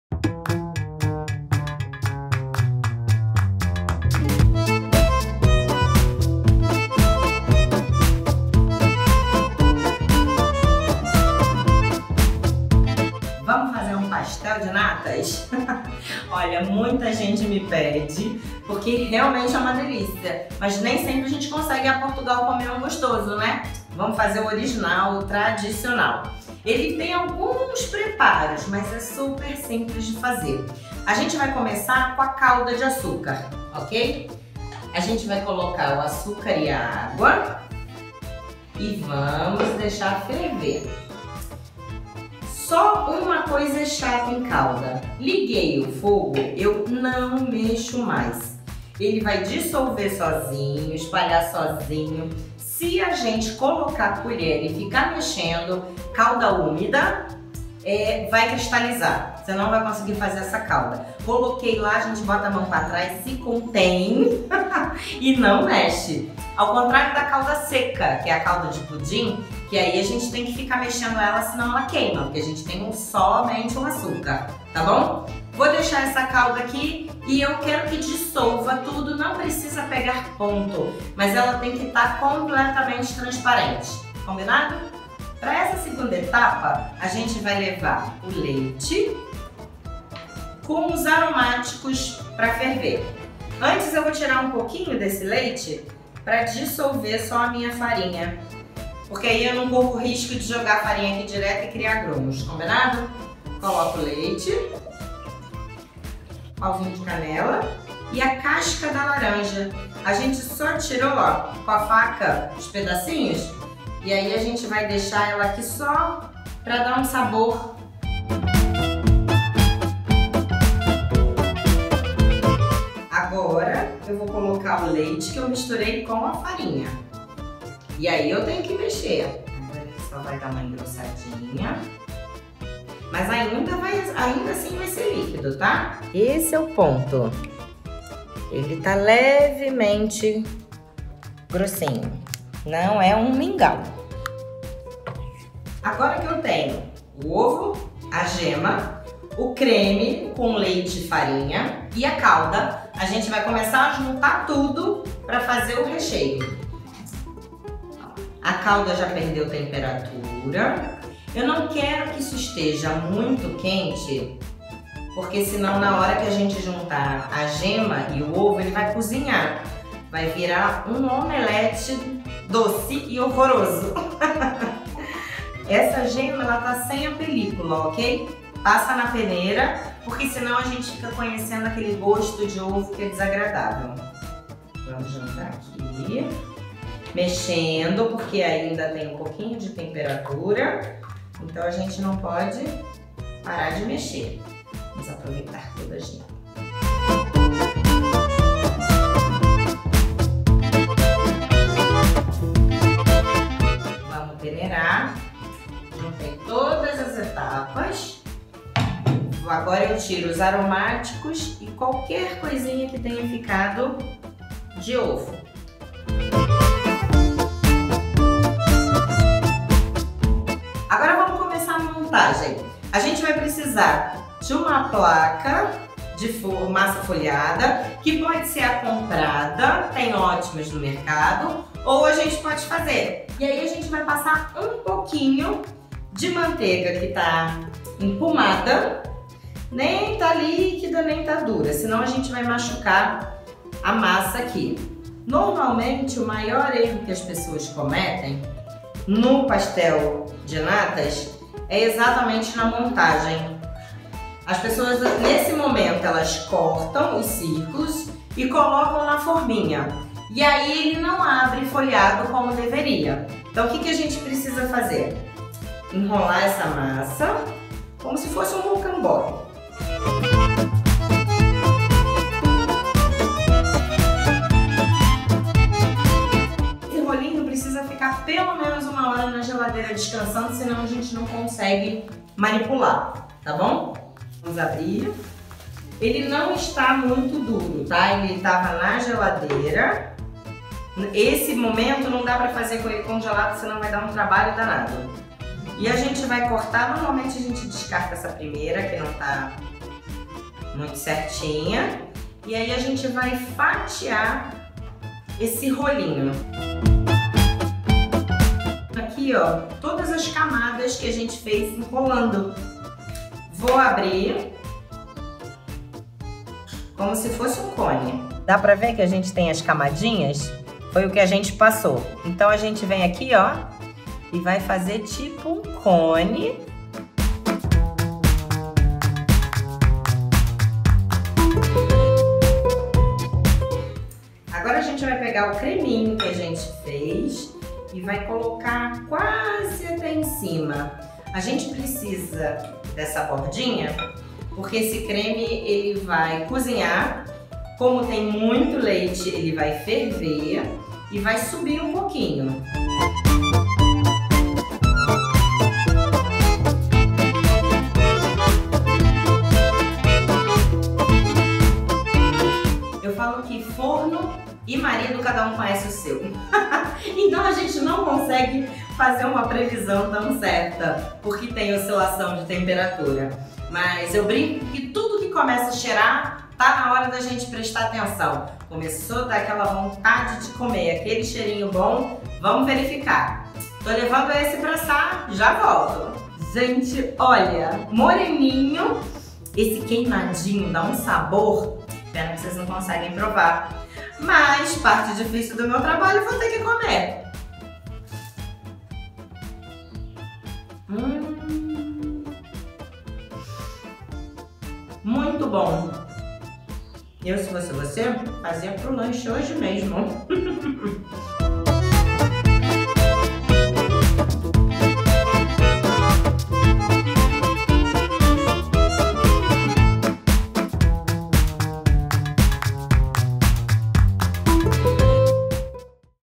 Pastel de Nata - Tá na Mesa
A chef Lu Neves ensina o modo de preparo do famoso Pastel de Nata. puxe uma cadeira.